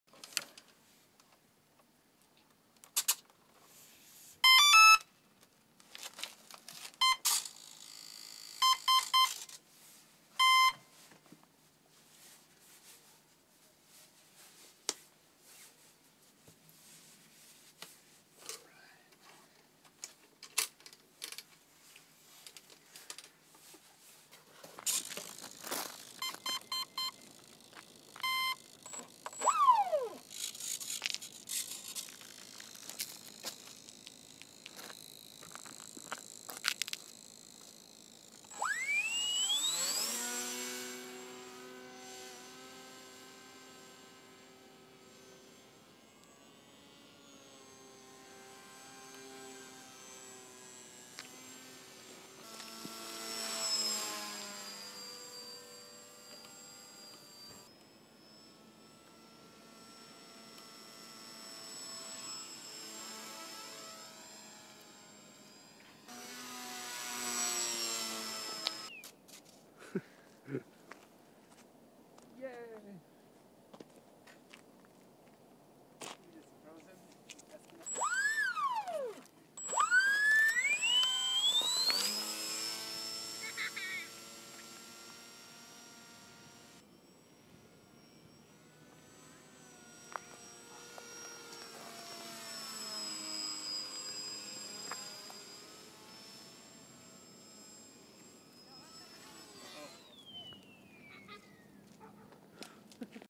Predator Drone RC Scratch Built Foamie
Predator Drone RC Scratch Built Foamie Prototype Number One

First flights of our Predator Drone - Electronics were mounted on the outside to allow for easy adjusting while we figure out the balance etc...

Plans will be available soon!